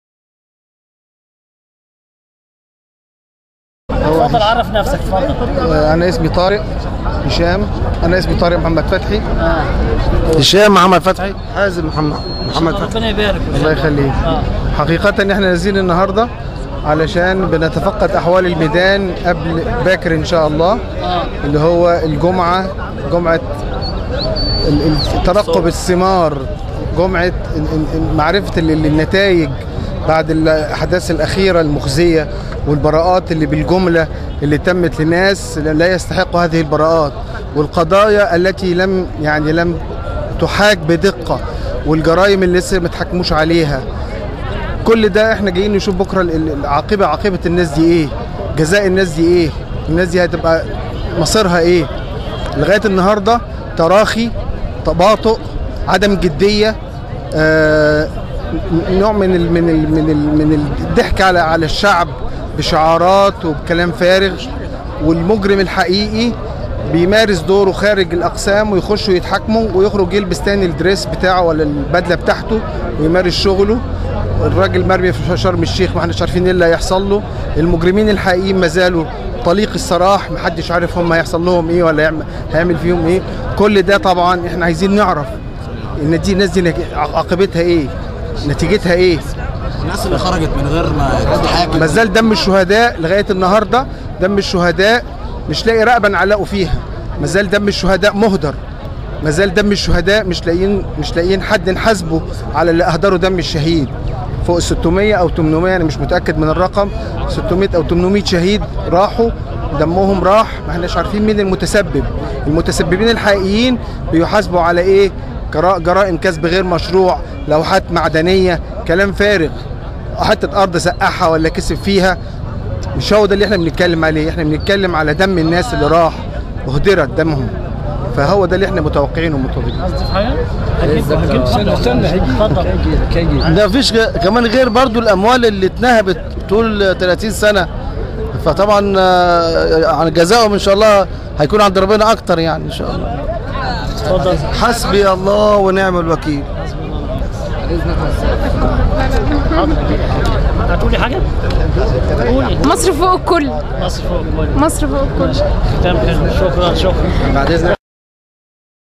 المتظاهرون يعلقون على اسلوب المحاكمة للنظام السابق
المتظاهرون يعلقون على اسلوب المحاكمة الخاطئة للنظام السابق و طرق بقايا النظام السابق